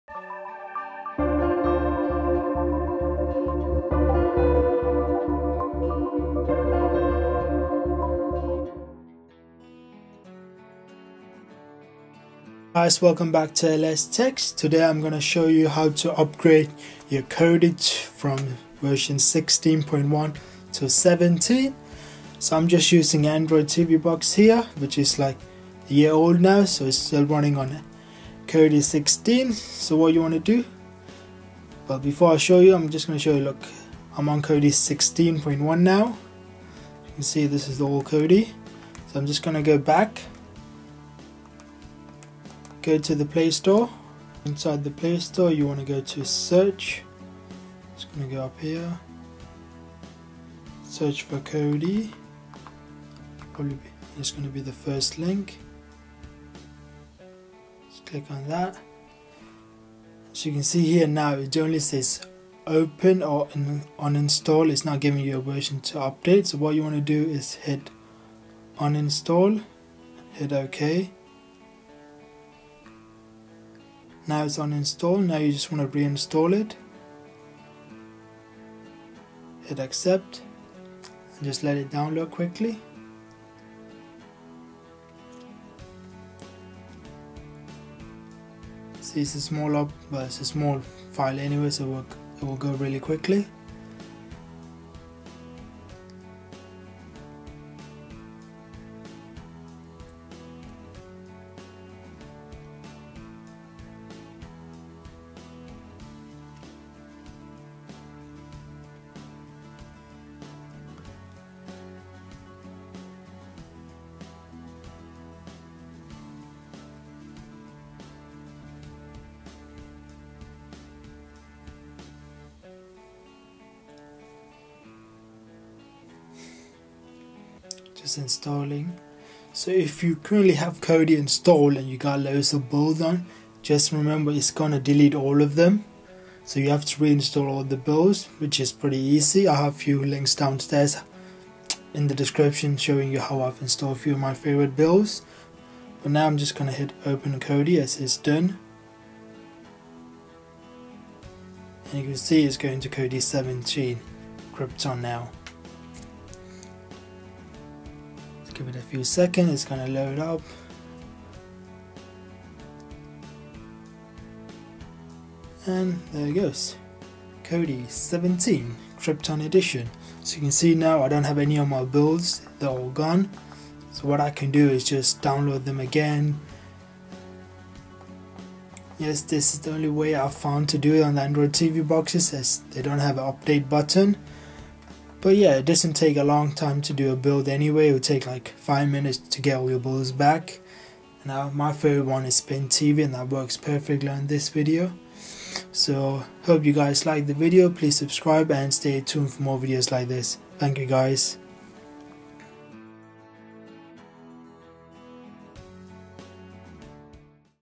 How to get Kodi 17 Krypton on Android Tv Box | Abox A1 Plus | Set Up
Hey guys, welcome back to LS Tech.
Today am going to show how to get Kodi 17 on Android Tv Box.

Amazon US
Amazon UK
Amazon DE

★ Amazon UK